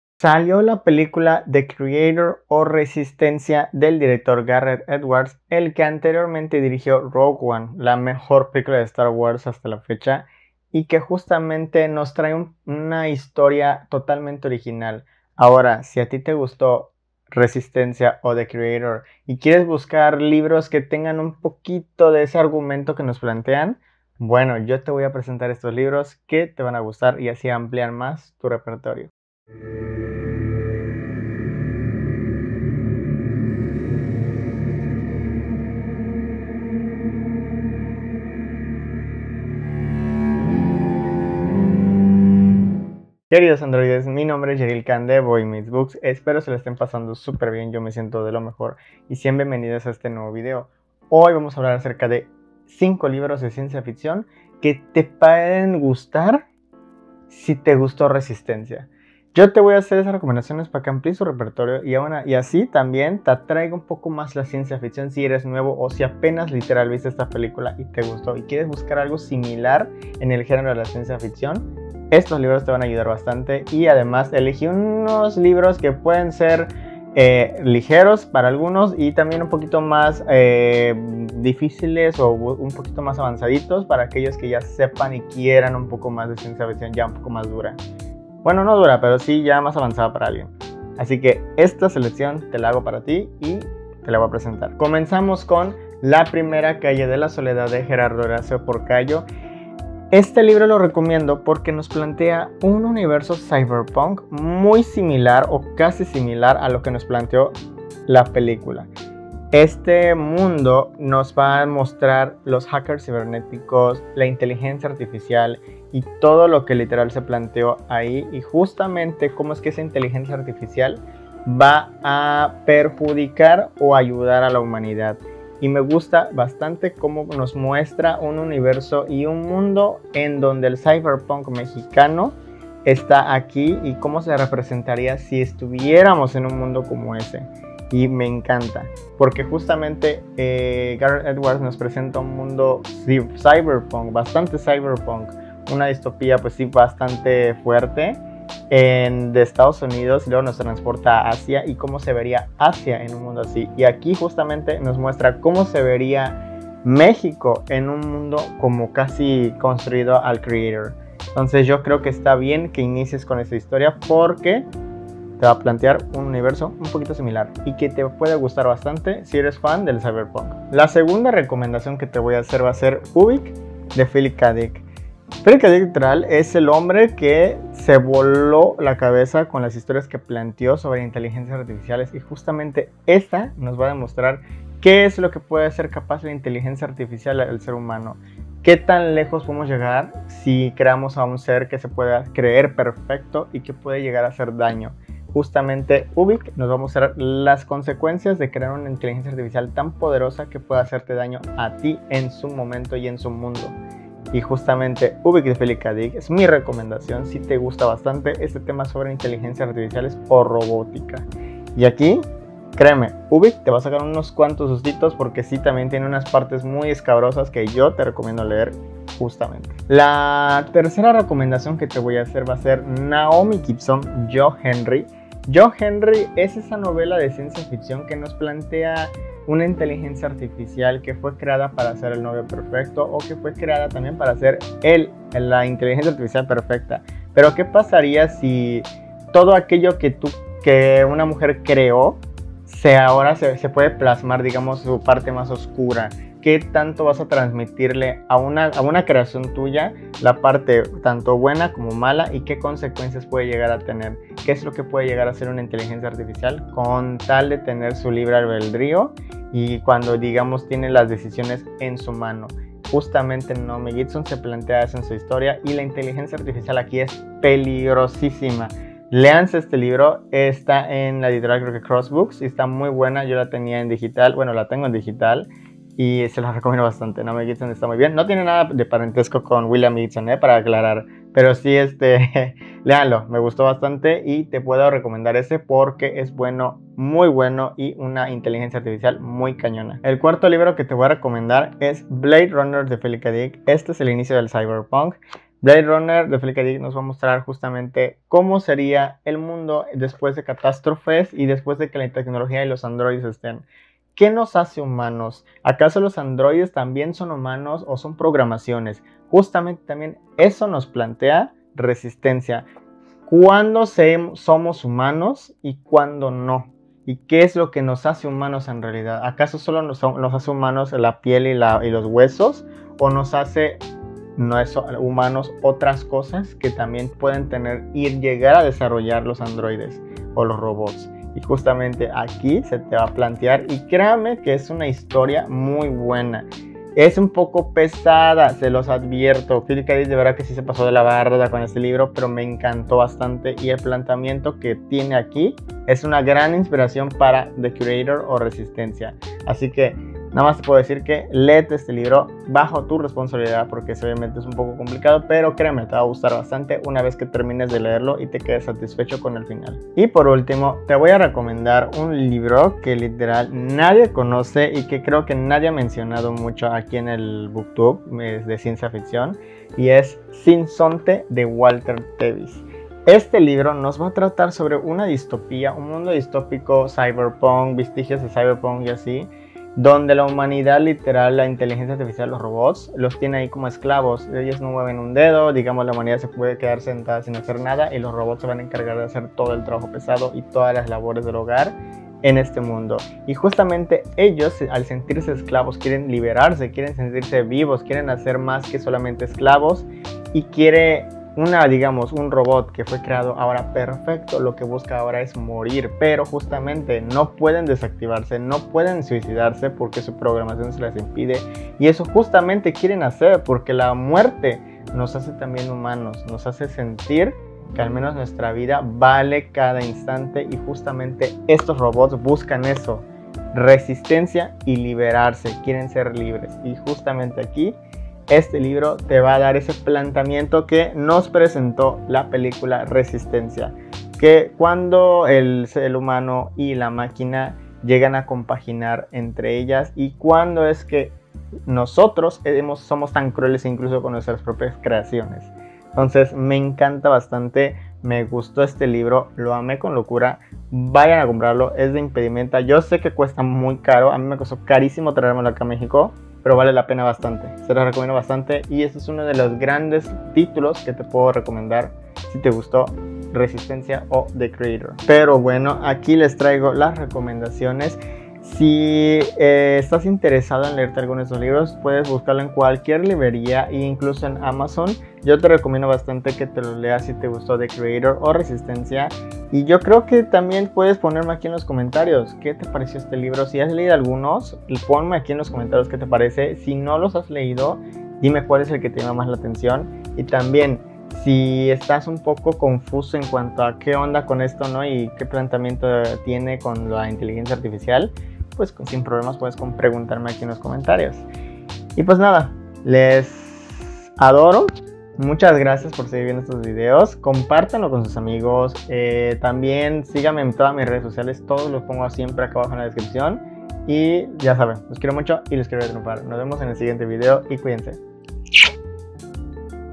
📖Si te gustó RESISTENCIA debes leer estos libros📖 | Boy Meets Books Mx
¡Hola mis androides! Hoy voy a recomendarles muchos libros que pueden gustarles si les gustaron otros.